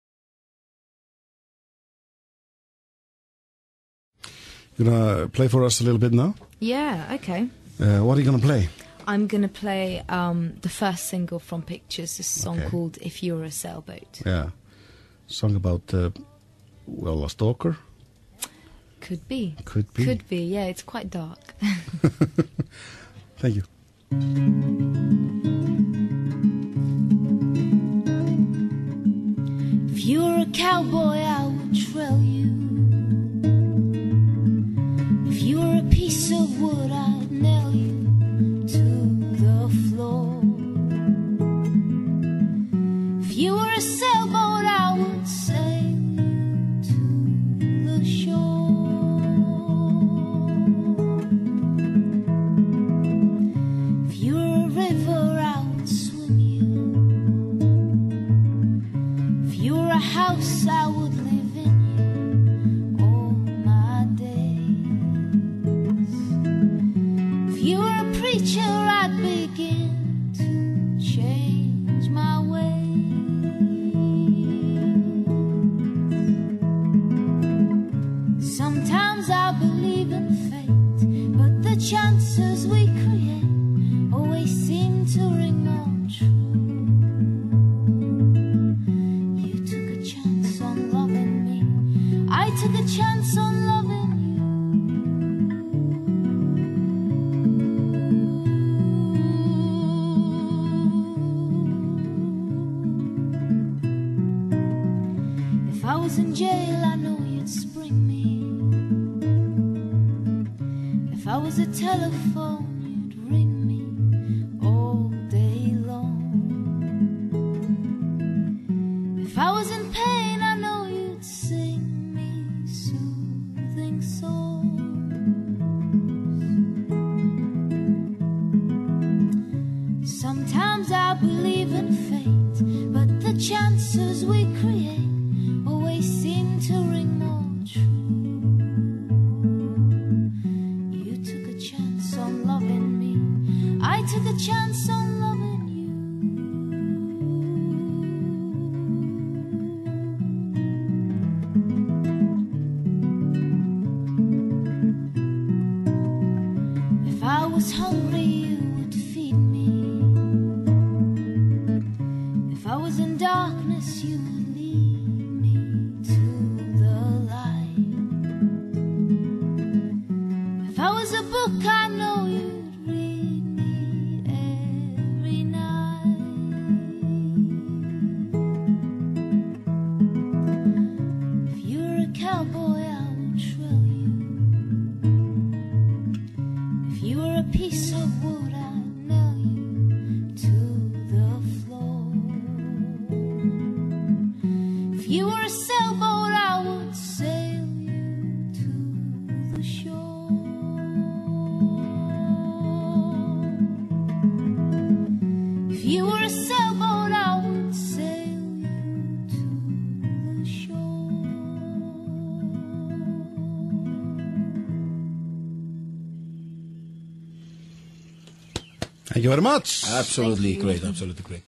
Katie Melua á Bylgjunni
Katie Melua kom í heimsókn í nóvember 2007. Hún var svo elskuleg að taka með sér gítarinn og taka lagið fyrir hlustendur.